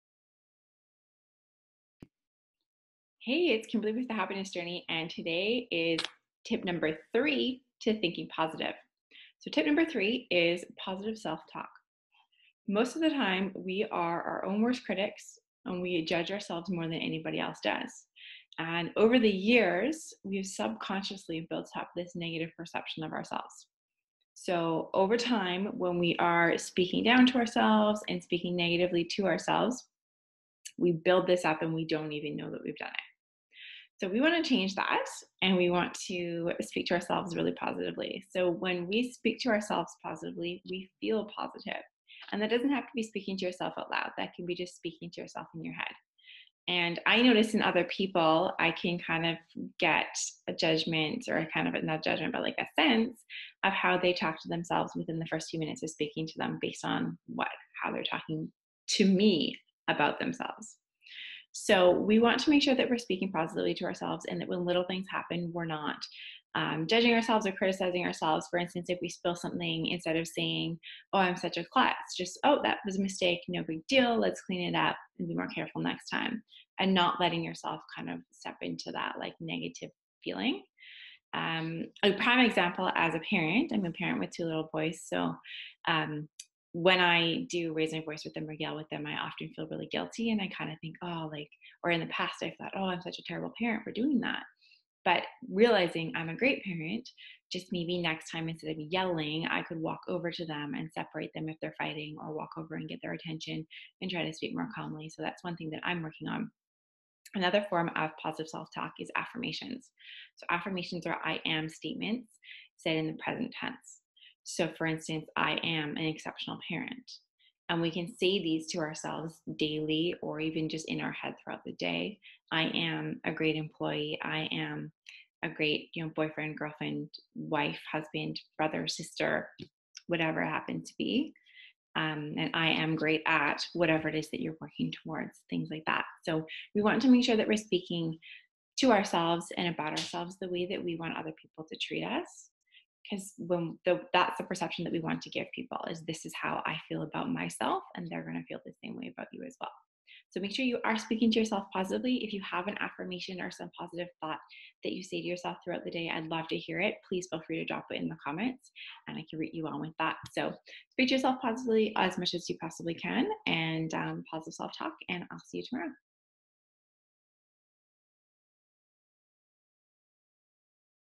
How to Think Positive: Positive Self-Talk
HOW TO THINK POSITIVE

This video is part of an 8 day series.

TIP #3
PRACTICE POSITIVE SELF-TALK

We tend to be our own worst critic. Over time, when we continue to criticize ourselves (regardless if our criticism is spoken aloud or in our head), we form a negative opinion of ourselves.

In order to change this we need to be aware of the voice in our head and respond with positive messages (self-talk).

Even a small shift in our self-talk can made a big change in the way we feel, as well as the way we think and behave under stress.

Examples:

Spill something?
Instead of “i’m such a klutz” try “oops, no big deal, i’ll be more careful next time”

Yelled at your kids?
Instead of “I’m a horrible parent” try “I get really upset, next time I will take a deep breath and stay calmer”

Frequently late?
Instead of “I suck” or “I just don’t have enough time” try “I’m going to leave 10 minutes early next time”

Self-talk can also appear in the form of affirmations. This is where we tell ourselves we are something that we WANT to become.

Affirmation examples:
I AM a calm parent
I AM an exceptional wife/husband/girlfriend/boyfriend
I AM great at organizing
I AM an exceptional employee
I AM great at managing my time

Treat yourself the way you want others to treat you.